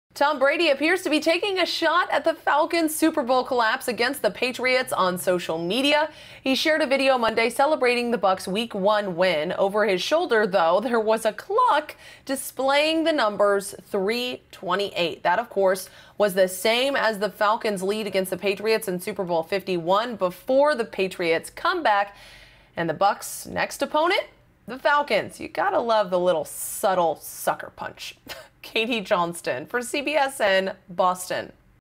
Tom Brady Subtly Trolls Falcons With Latest Video On Social Media
Tom Brady likes his social media. Whether he's pushing some products, making fun of himself, or sharing a good hype video, the man's got all the channels working at all times. Katie Johnston reports.